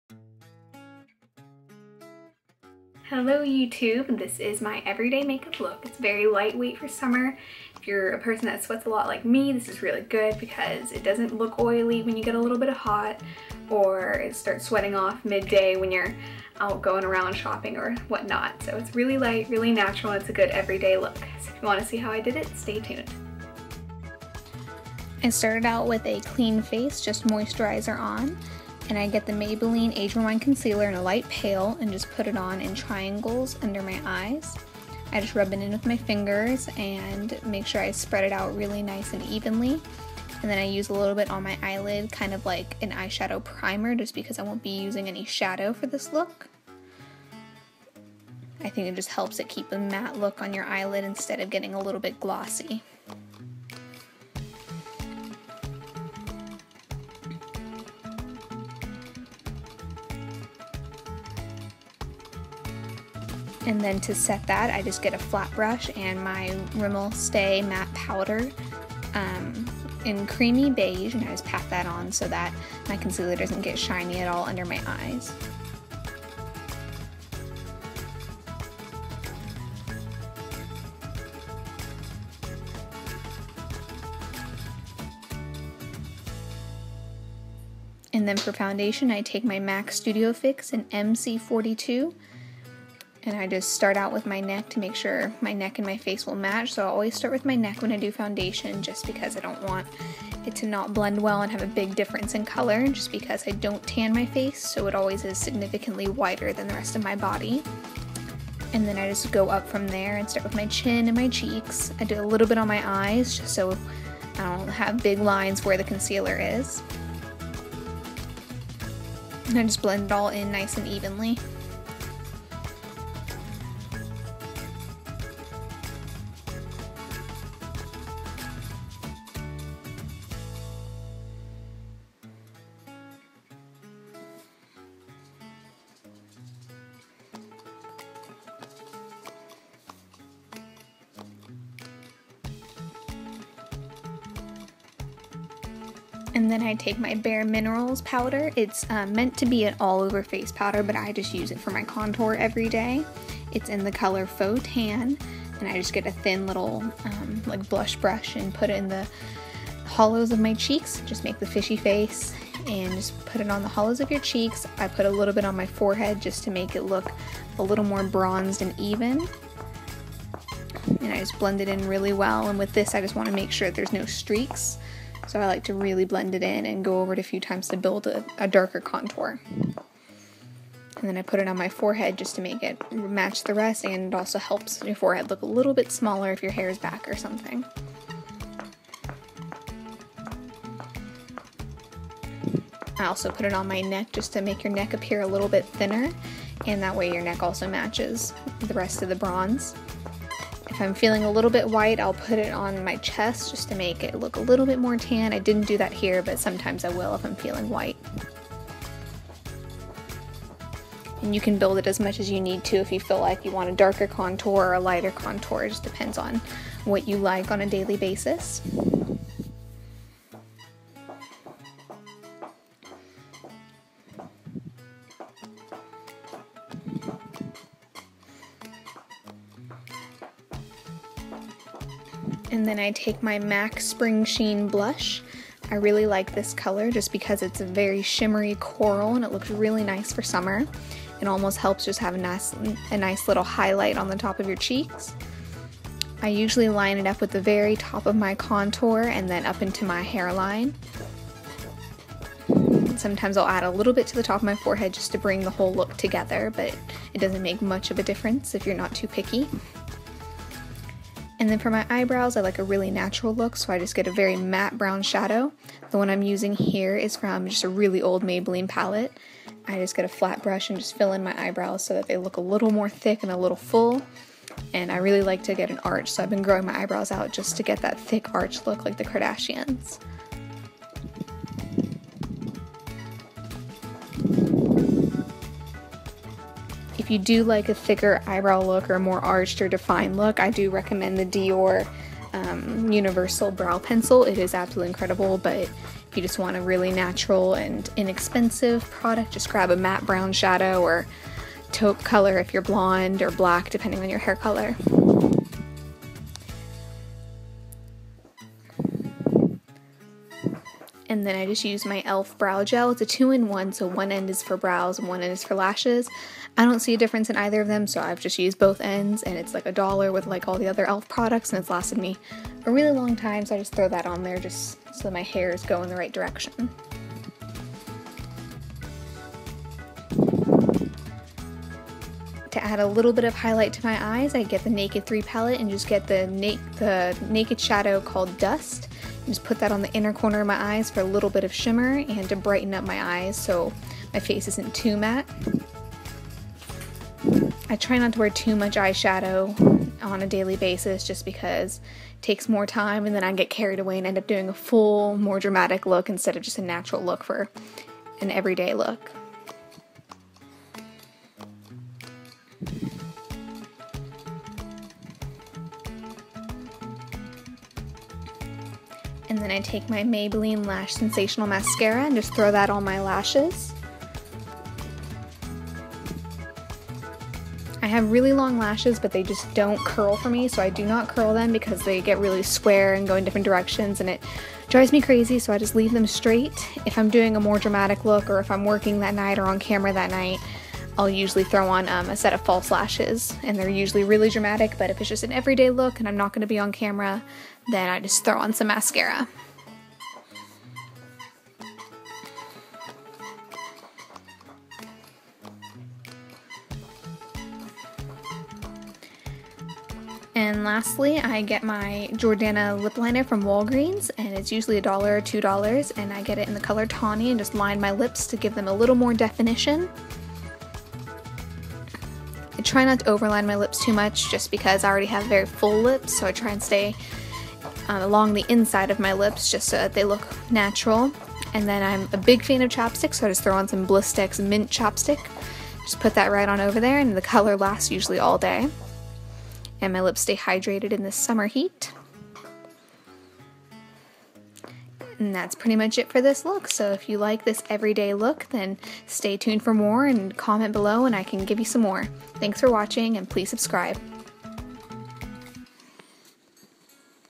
Easy Everyday Natural Look
What I used:
Maybelline Age Rewind Concealer: Light Pale
Rimmel London Stay Matte Powder: Creamy Beige
Mac: Studio Fix NC 42
Bare Minerals: Faux Tan Powder
Mac: Springsheen Blush
Maybelline Eyeshadow: Idk the name, it's really old. Just a matte brown shadow.
Elf: Clear Brow/Lash Gel
Maybelline: Lash Sensational Mascara
Urban Decay: Naked 3 Dust